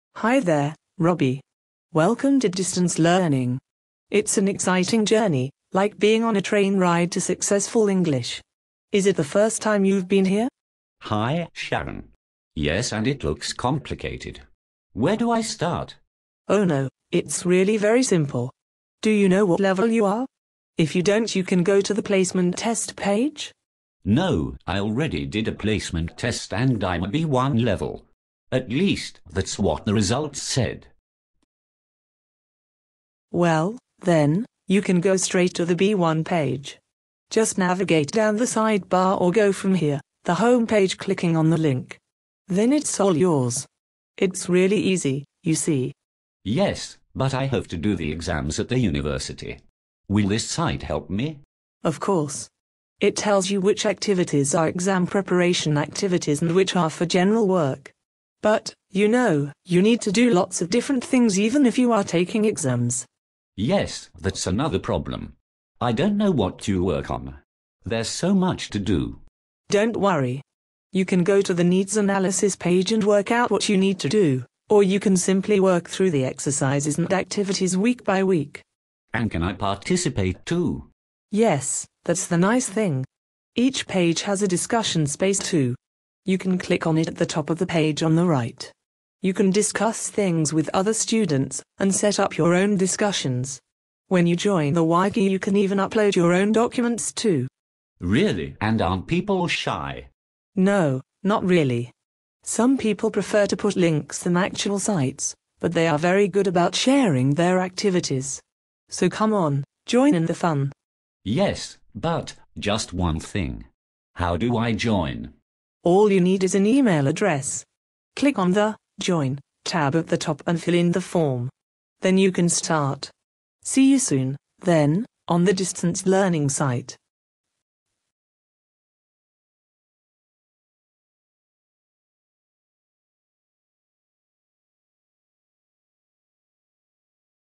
Welcome to distance learning
This video is a short walkthrough explaining some ways to use this wiki. See you there, I hope.